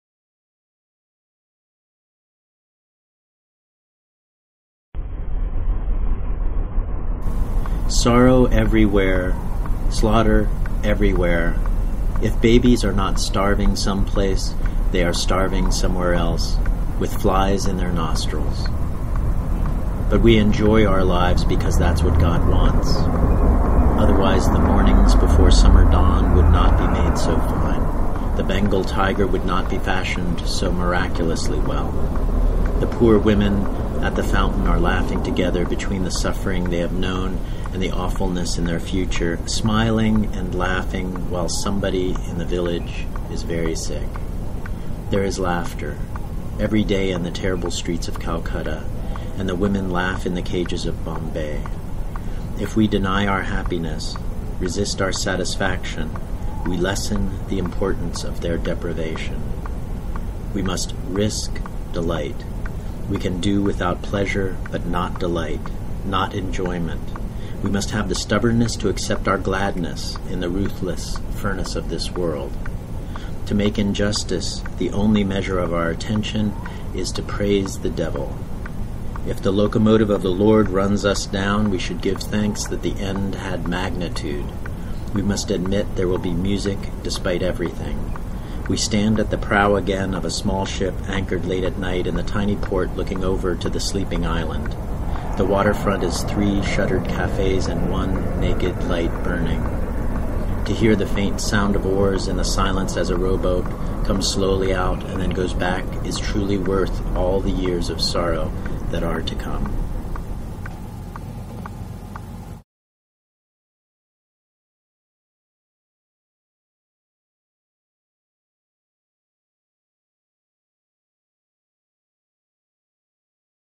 Jack Gilbert - "A Brief for the Defense"
John E. reads Jack Gilbert's "A Brief for the Defense" as part of DIESEL Bookstore's celebration of National Poetry Month.  For the month of April, DIESEL will be posting a new video each day.  Enjoy!